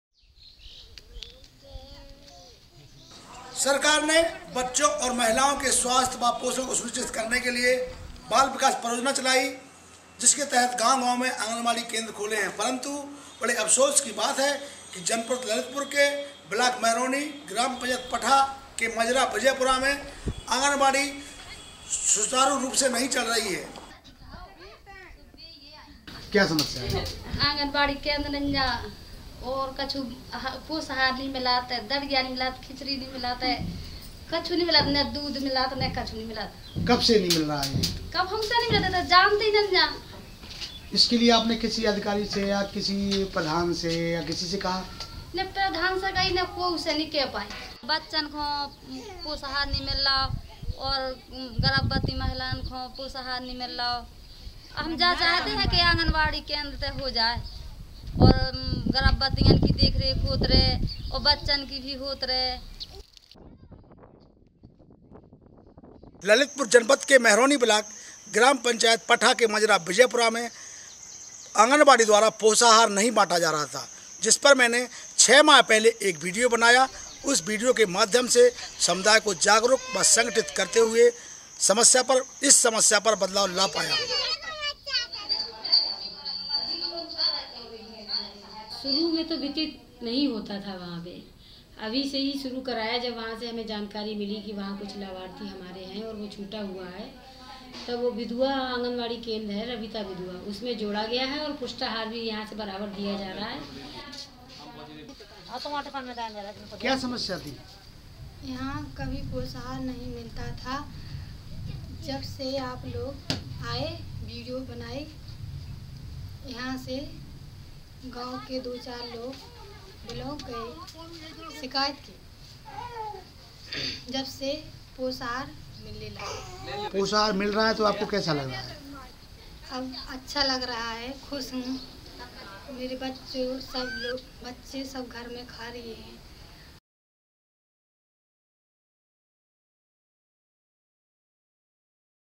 Impact: Anganwadi starts serving food
Community Correspondent Shankar Lal reports from Lalitpur District, Uttar Pradesh for Video Volunteers.